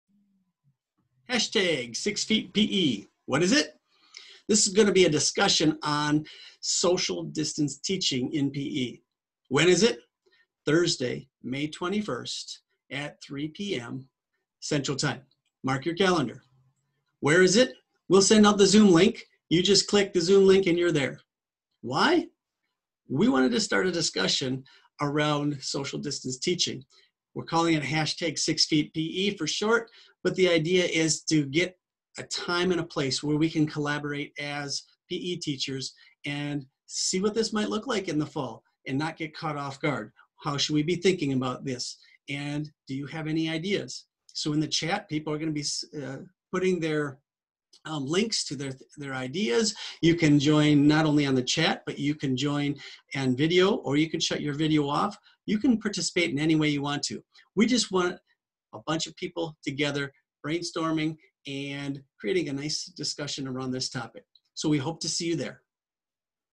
6feetPE
Come join the discussion around teaching from 6 feet apart.  We go live on Thursday 3:00 pm CT